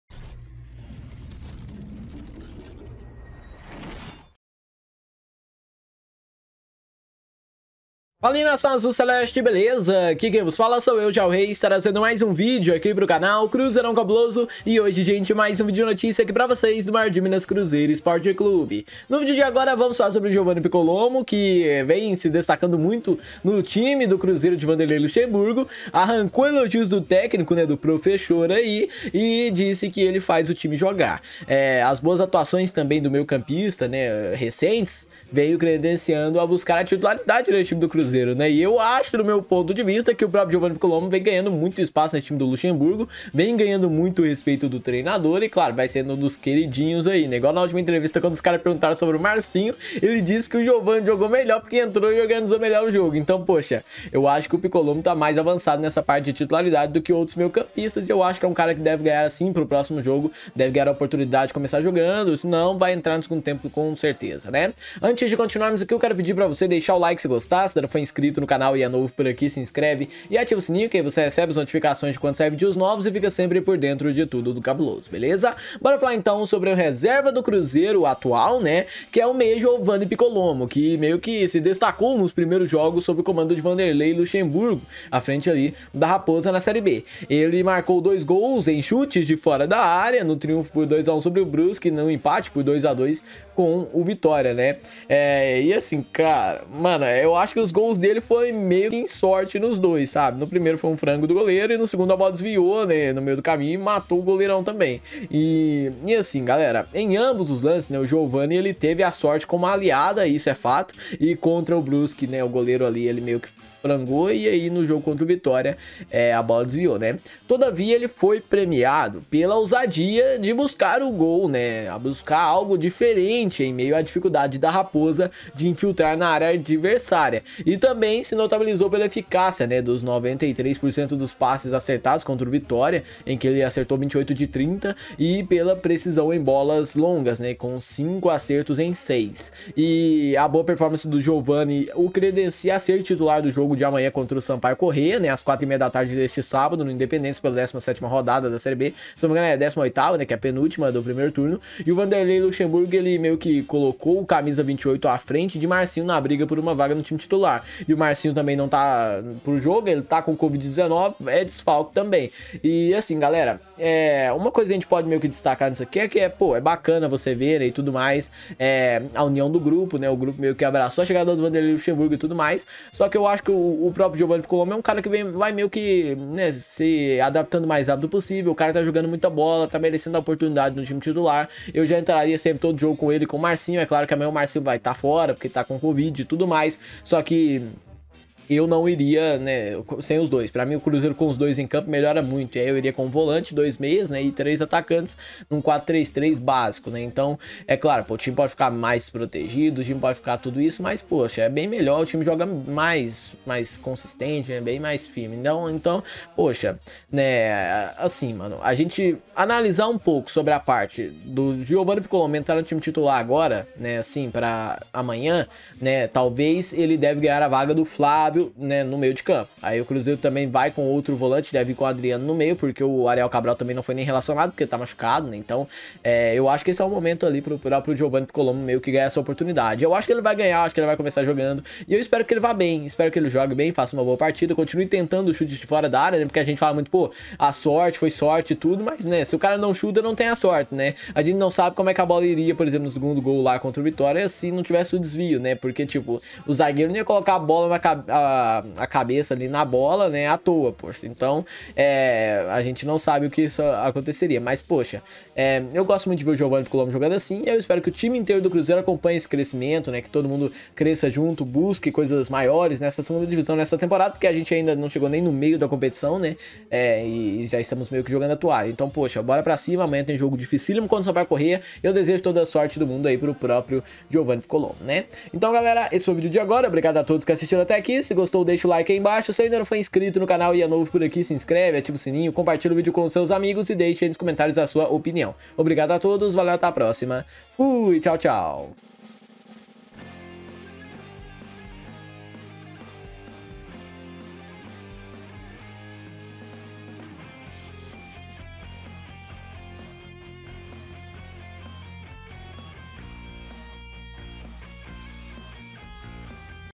Giovanni arranca elogios de Luxemburgo no Cruzeiro !!!
Giovanni arranca elogios de Luxemburgo no Cruzeiro: 'Faz o time jogar'
Boas atuações recentes credenciam armador a buscar titularidade
Reserva no Cruzeiro, o meia Giovanni se destacou nos primeiros jogos sob o comando de Vanderlei Luxemburgo na Série B. Ele marcou dois gols em chutes de fora da área no triunfo por 2 a 1 sobre o Brusque, no último sábado, em Santa Catarina, e no empate por 2 a 2 com o Vitória, nessa quarta-feira, no Independência.
Em ambos os lances, Giovanni teve a sorte como aliada. Contra o Brusque, o goleiro Zé Carlos se atrapalhou ao tentar defender uma bola aparentemente fácil e espalmou para dentro do gol. Diante do Vitória, o volante uruguaio Pablo Siles desviou de cabeça e tirou o goleiro Lucas Arcanjo de cena.

Todavia, Giovanni foi premiado pela ousadia de buscar algo diferente em meio à dificuldade da Raposa de infiltrar na área adversária. Também se notabilizou pela eficácia de 93% nos passes contra o Vitória (28 em 30) e pela precisão em bolas longas (cinco em seis).

A boa performance de Giovanni o credencia a ser titular no jogo contra o Sampaio Corrêa, às 16h30 de sábado, no Independência, pela 17ª rodada da Série B. Vanderlei colocou o camisa 28 à frente de Marcinho na briga por uma vaga no time.

“É um jogador que já vem entrando bem na equipe. Ele faz o time jogar. Se você pegar o Giovanni, ele conseguiu clarear muito mais as jogadas que o Marcinho. Ele conseguiu achar os jogadores e os colegas”, disse.

“Por exemplo, entre Marcinho e Giovanni, eu fico com Giovanni que fez a equipe jogar muito mais. Ah, o Marcinho entrou bem, mas não entrou como deveria entrar, dando mais velocidade, tapa na bola, chegando à linha de fundo e achando os colegas”, completou.

Giovanni começou entre os 11 iniciais pela última vez no empate por 3 a 3 com o Botafogo, em 10 de julho, no estádio Nilton Santos, no Rio de Janeiro. Na ocasião, o armador cometeu um pênalti aos 47 minutos do segundo tempo - Chay converteu a cobrança e igualou o marcador.

Nos compromissos posteriores, Giovanni entrou no segundo tempo sob o comando do então técnico Mozart Santos, que pediu demissão após o empate por 2 a 2 com o Londrina, no Mineirão, pela 15ª rodada. Com a chegada de Luxemburgo, o armador foi bem individualmente e enfim pode reaparecer na equipe principal.

Caso confirme a opção por Giovanni, Luxemburgo deverá tirar Flávio, que saiu justamente no intervalo da partida de quarta-feira. O treinador comanda o último treino do elenco nesta sexta, quando definirá a lista de convocados visando ao confronto com o Sampaio Corrêa no Independência.